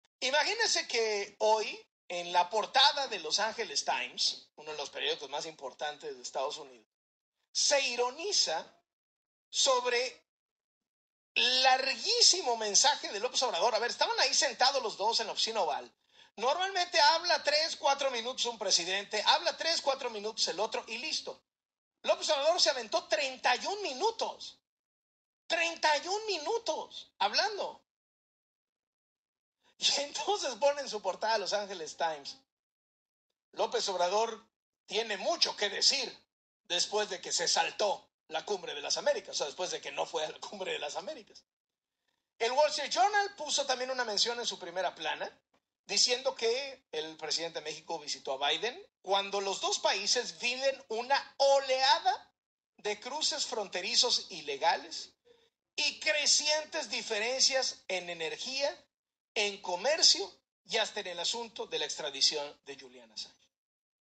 ¡TODOS LOS PERIÓDICOS DE USA SE BURLAN DE AMLO! SUS DÍAS ESTÁN CONTADOS! FUE LA BURLA DE EE.UU.
AMLO fue una burla en todos los periódicos estadounidenses en donde se burlan del fracaso de la reunión de este con Joe Biden Presidente de los Estados Unidos de América